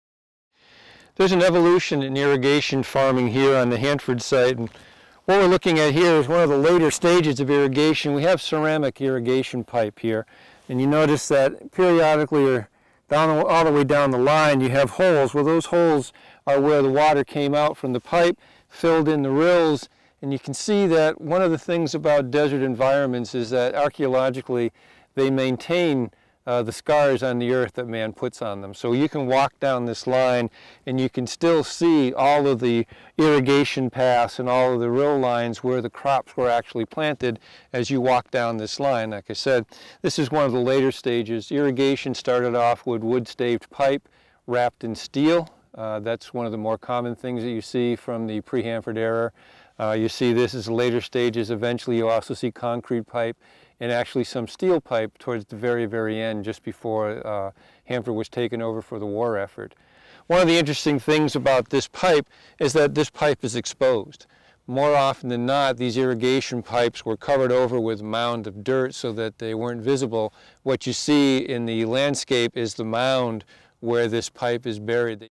Irrigation Evolution
Mission Support Alliance archaeologist Tom Marceau describes pre-war irrigation farming on the Hanford Site, focusing on an exposed ceramic irrigation pipe.